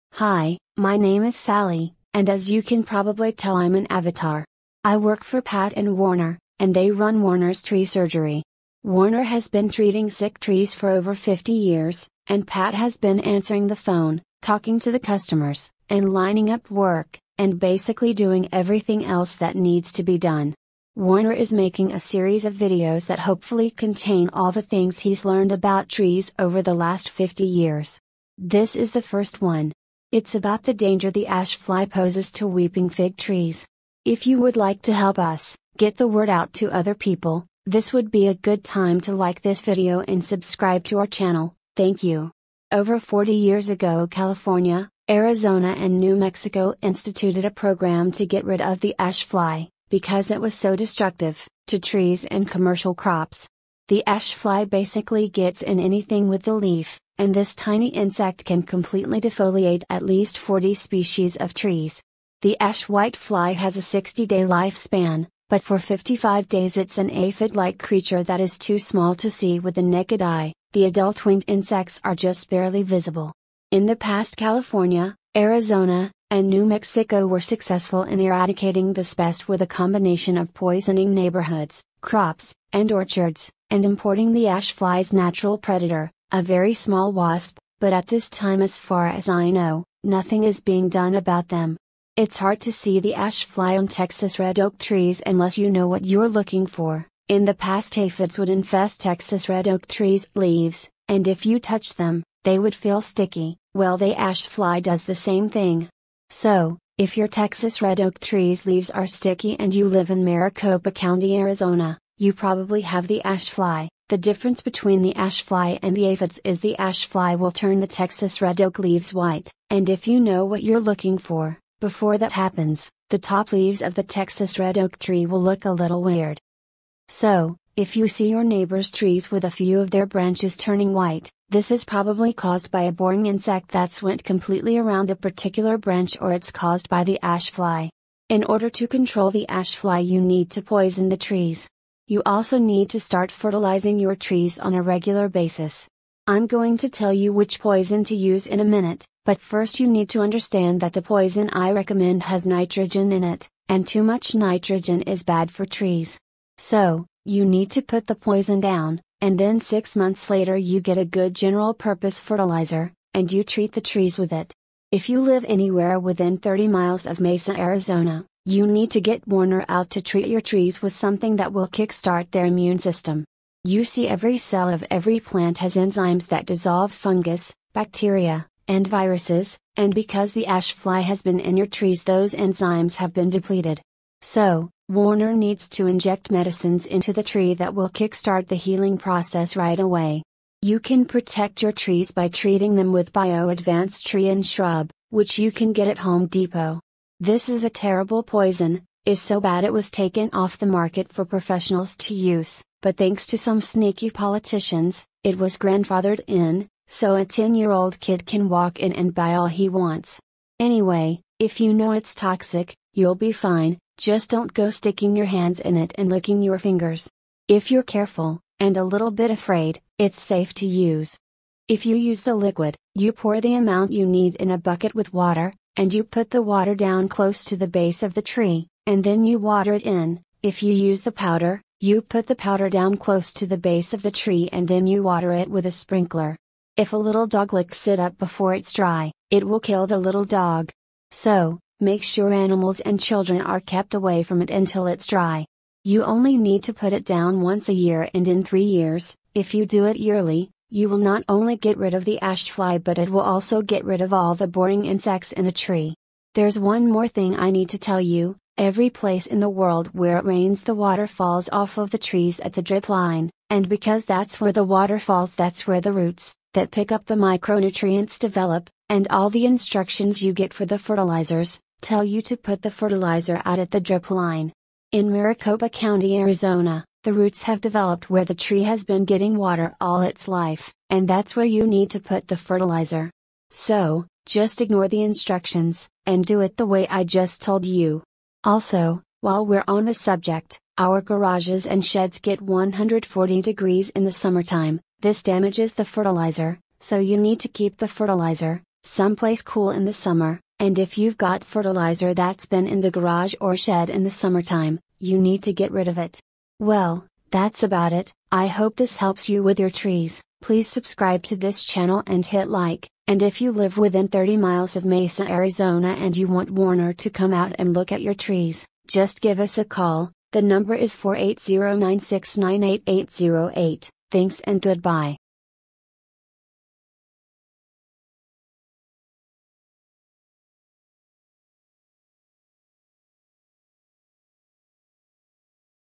50 Years a Tree Doctor Unveiling the Secrets of Weeping Fig Diseases
Join me, a seasoned arborist with over half a century of experience, as I delve into the mysterious world of Weeping Fig diseases. In this video, I’ll share my expert insights on identifying, treating, and preventing ailments that plague these majestic trees. Discover the intricate balance of nature and learn how to keep your Weeping Figs thriving despite the challenges they face.